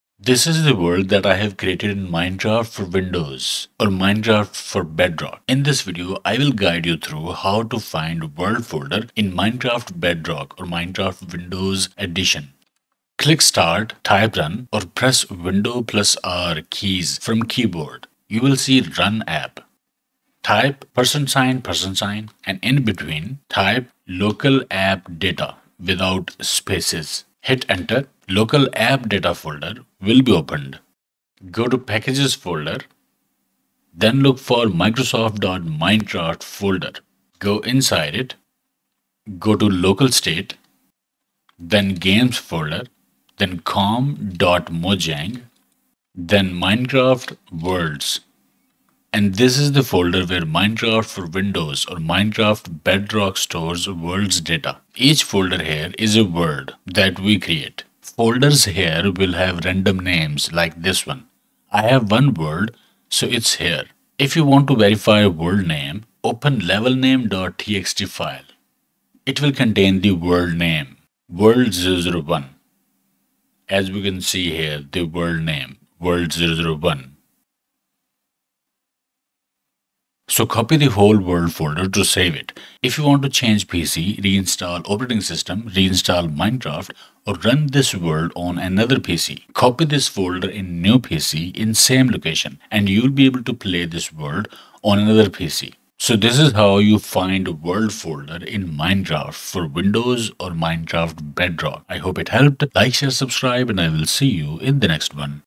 How to Find World Folder in Minecraft Bedrock
In this video I am gonna guide you how to find world folder in Minecraft Bedrock edition. Minecraft for Windows saves world files at different location than Minecraft Java edition. This tutorial will help you locate world files for Minecraft Windows.